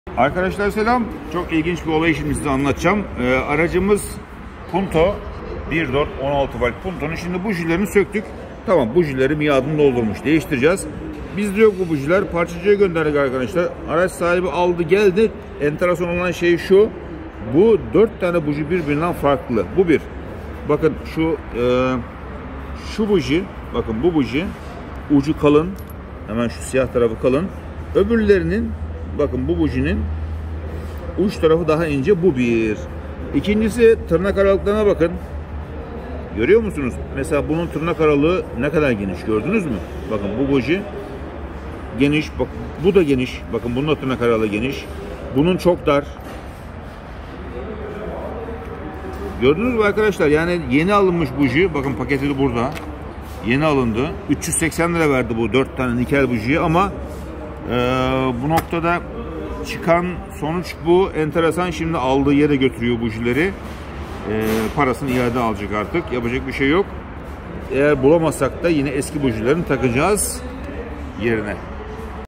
Yok Artık ;Yeni Buji Aldı Sahte Çıktı ( iade)
Fiat Punto aracına 0 buji alan müşterimiz gördüğü tablo  karşısında şok oldu çünkü bujiler sahte